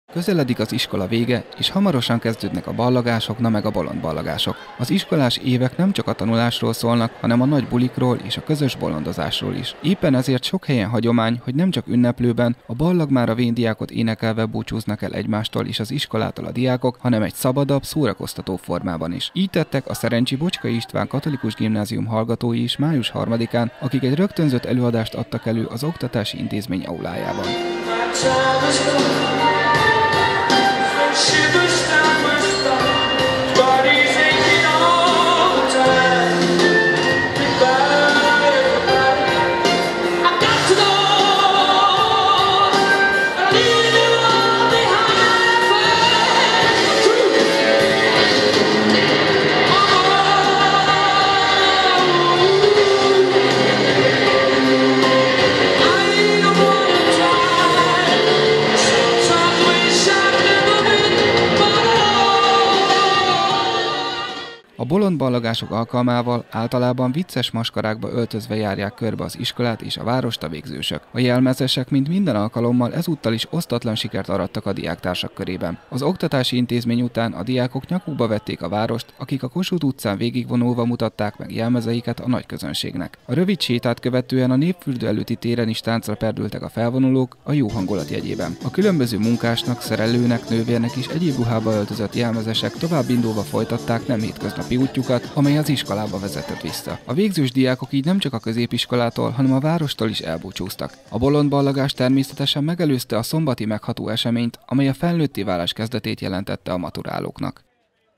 BOLONDOZVA BALLAGTAK A BOCSKAIBAN
Bolondos módon, énekkel, tánccal és a legkülönbözőbb jelmezekkel búcsúztatták május 4-én a hamarosan véget érő diákéveket a szerencsi Bocskai István Katolikus Gimnázium tanulói. A legtöbb fiatal spontán ötlettől vezérelve öltözött be, de akadtak olyanok is, ahol előre kitalált jelmezt húztak magukra. Volt olyan, aki rendőrnek, tűzoltónak, angyalnak, parasztlánynak, utcaseprőnek, vagy éppen szerelőnek sejknek öltözött be. A végzős diákok így nem csak a középiskolától, hanem a várostól is elbúcsúztak.